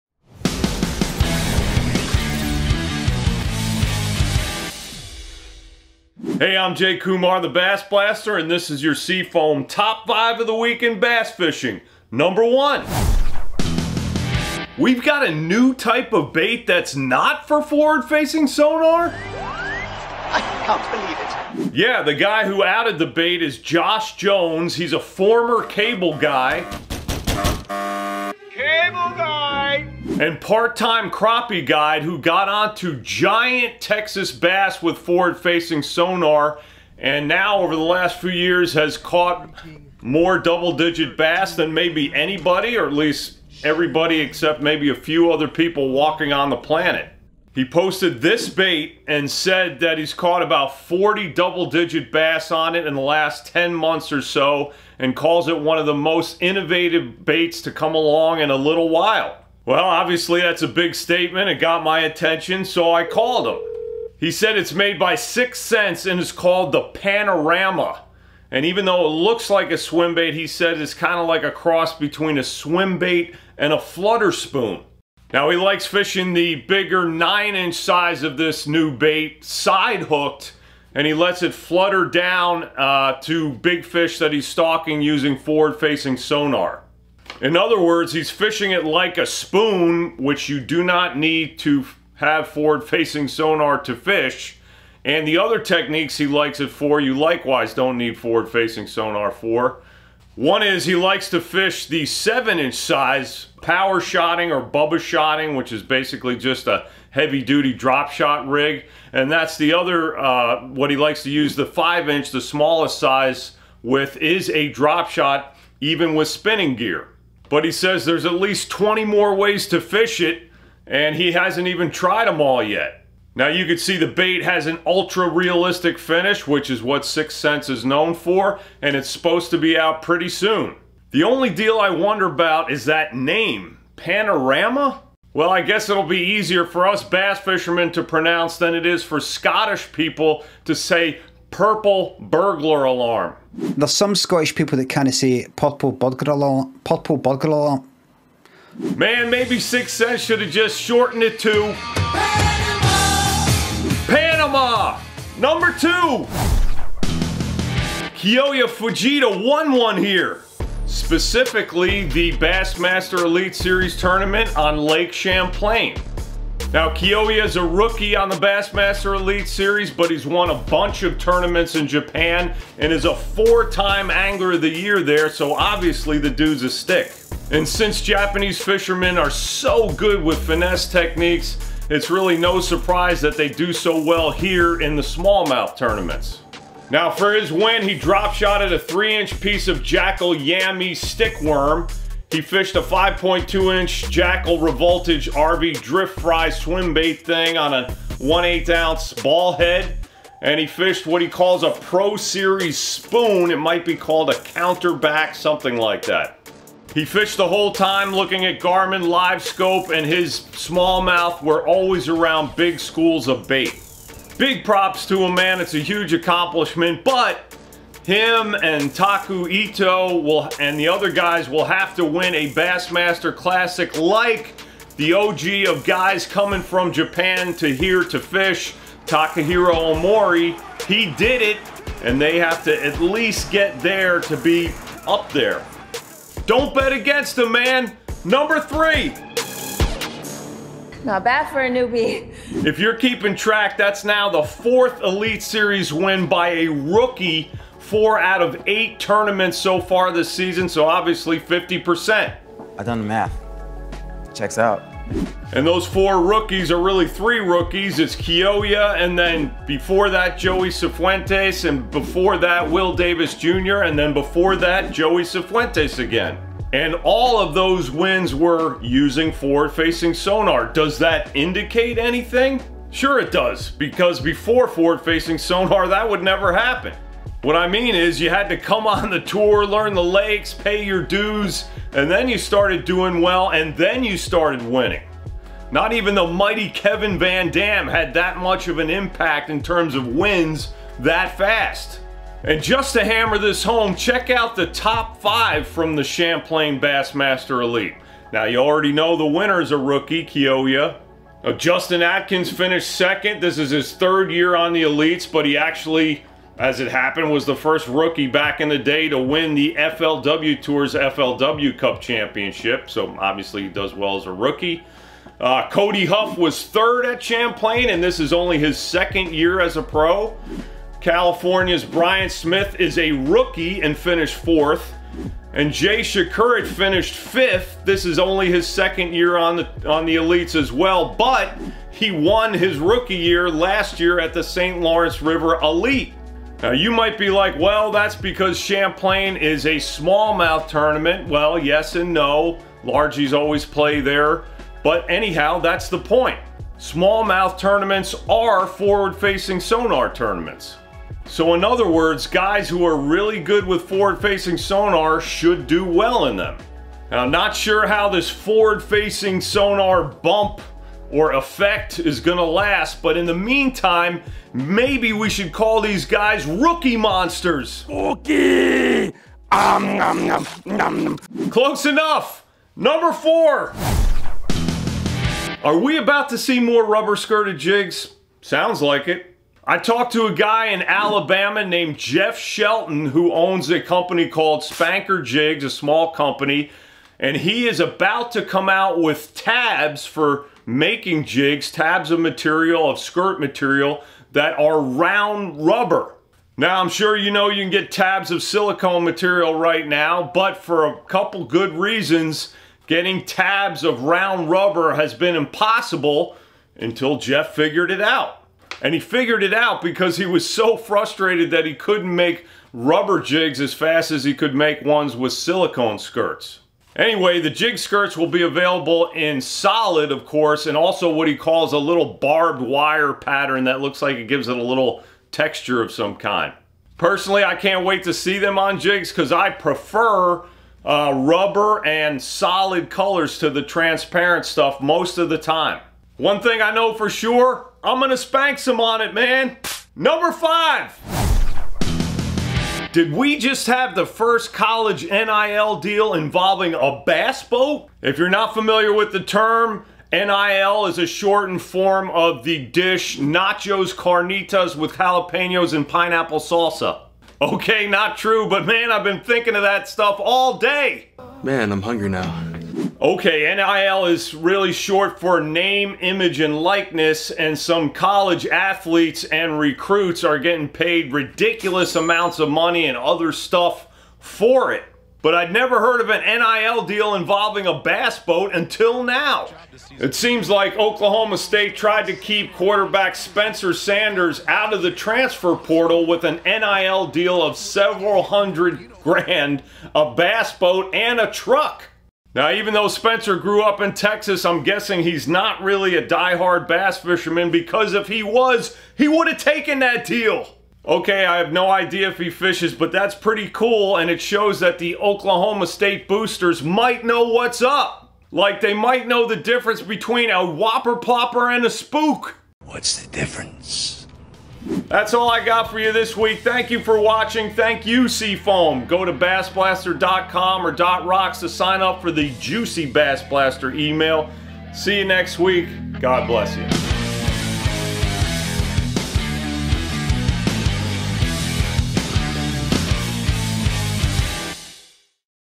New bait type? Rookie wins with FFS, Bass boat NIL deal? Top 5 of the Week in Bass Fishing Ep 62
So we have a new bait type that's NOT just for forward-facing sonar (FFS)? 🤯
Kyoya Fujita got his first win over here! (He's been a stick in Japan!)
Rookies are and have been winning with FFS – how long will it last? 🤔
Are more rubber-skirted jigs 'n such coming? 👀
The first Bass Boat college NIL deal?

Got all that in this week's SeaFoamWorks Top 5 in Bass Fishing, Episode 62! 🎯

That way YouTube likes the videos more! Thanks much! 👈

Learn more about the amazing Sea Foam products 👉 SeaFoamWorks.com